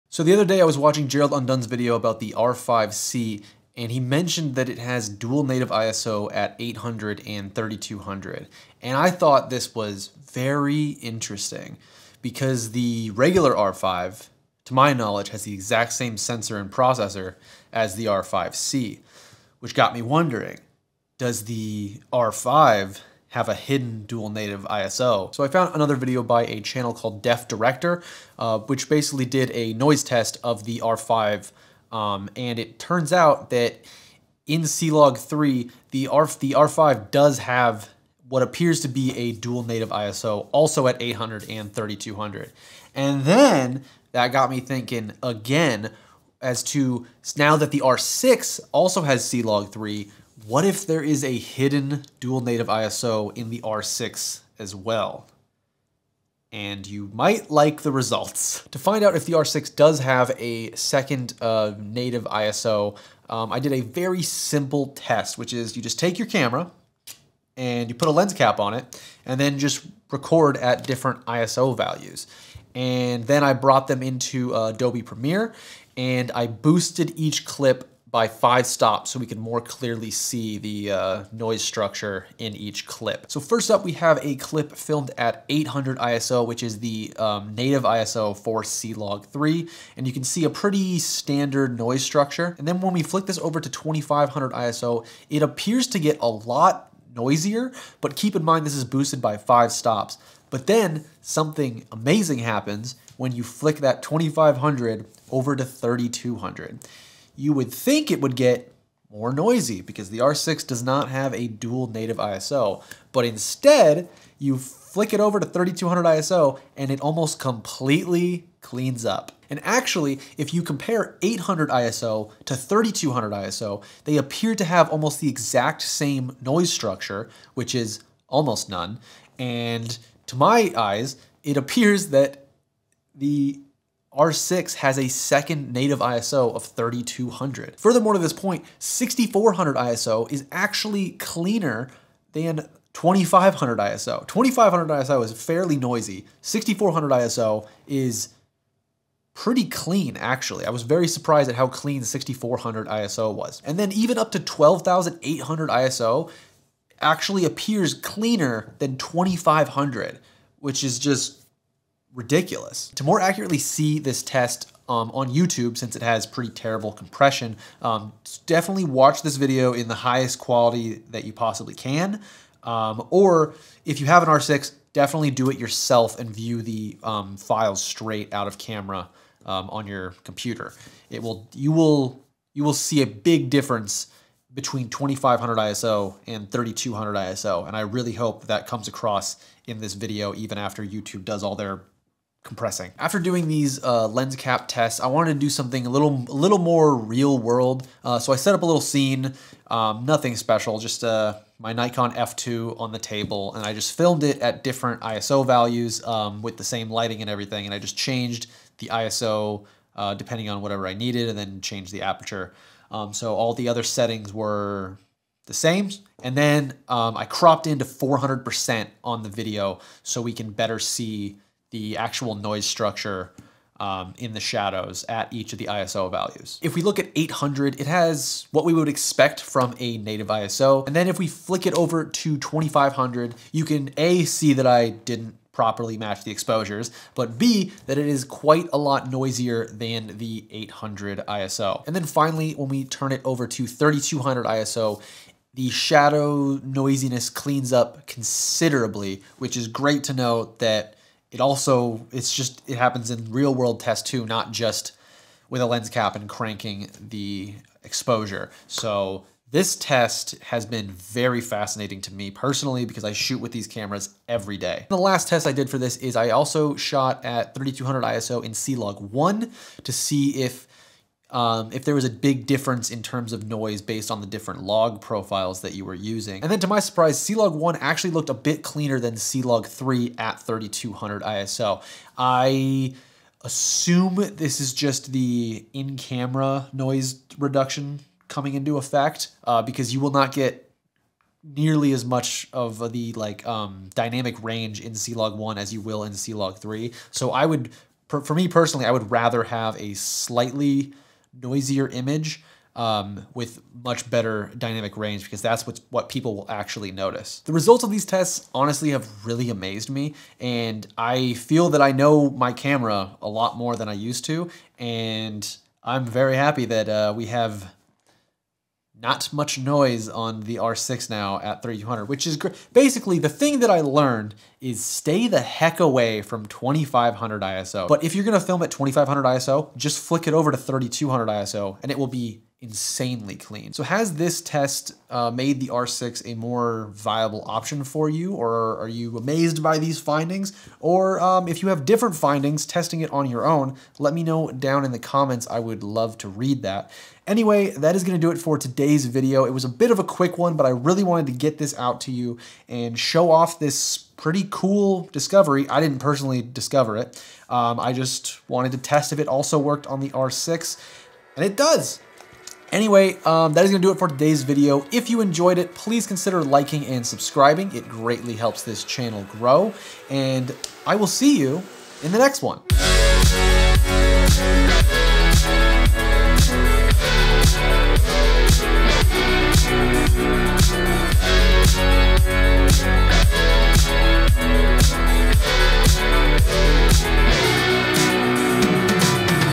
Does the Canon R6 have DUAL NATIVE ISO?!?!?
Today I did a little experiment to see if the Canon R6 has a hidden dual native ISO and to my surprise, it seems to have it!

Canon EOS R6
Canon 24mm f1.4 L II
Canon 85mm f1.4 IS
Canon 16-35mm f2.8 III
Rode Videmic Pro +
LowePro Pro Tactic
Canon EF-RF Drop In Filter
Prograde UHS II V90 Cards